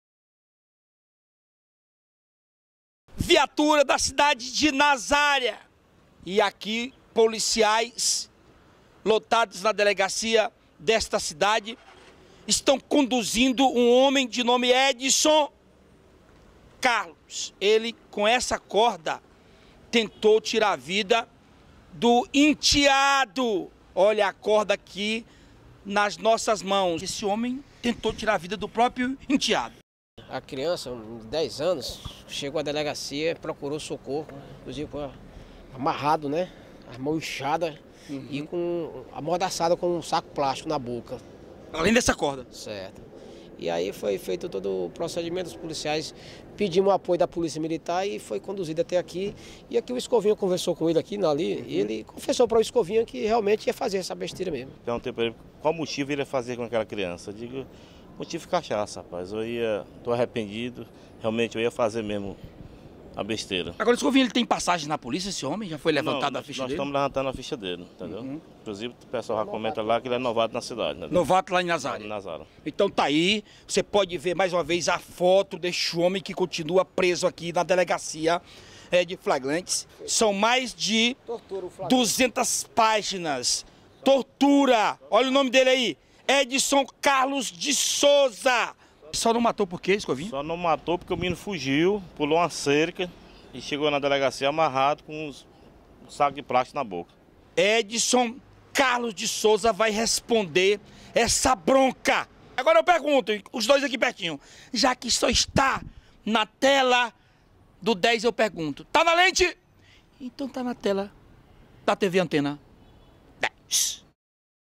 PADRASTO É ACUSADO DE TENTATIVA DE HOMICÍDIO
PADRASTO É ACUSADO DE TENTAR MATAR O ENTEADO DE APENAS 10 ANOS DE IDADE NA CIDADE DE NAZÁRIA. ELE FOI PRESO E LEVADO PARA A CENTRAL DE FLAGRANTES.